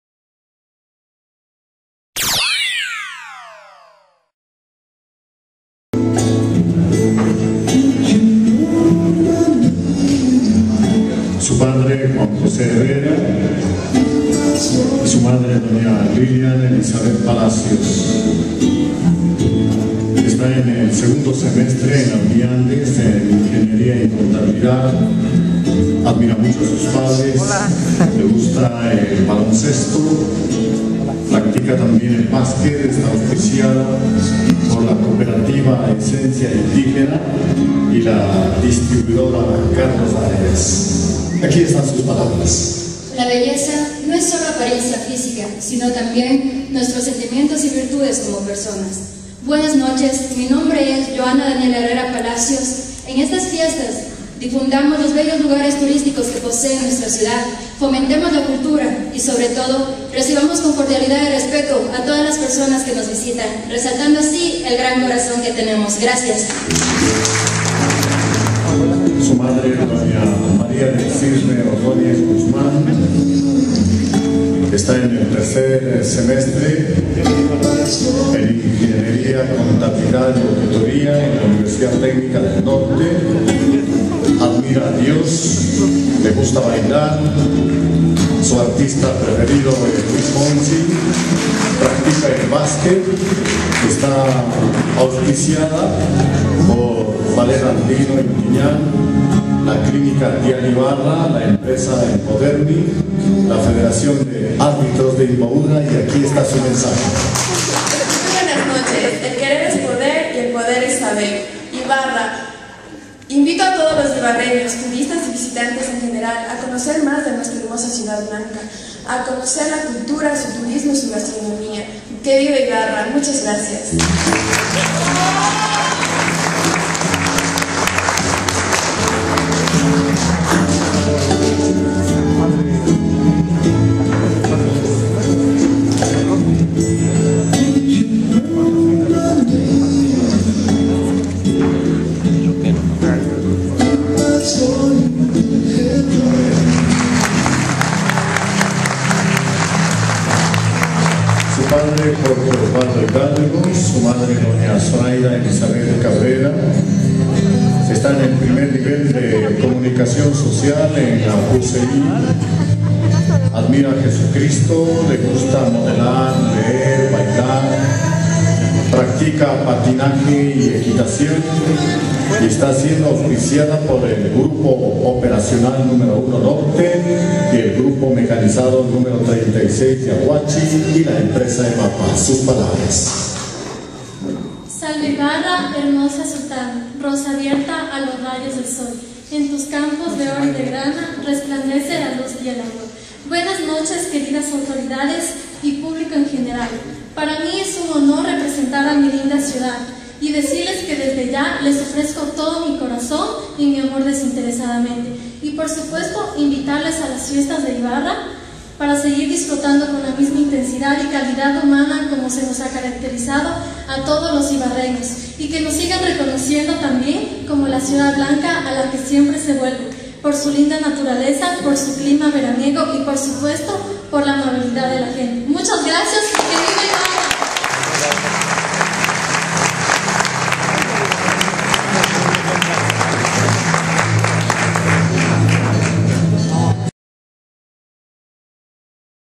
PRESENTACIÓN CANDIDATAS A REINA DE IBARRA.wmv
Las siete candiatas se presentaron oficialmente a los medios de comunicación y a los invitados especiales.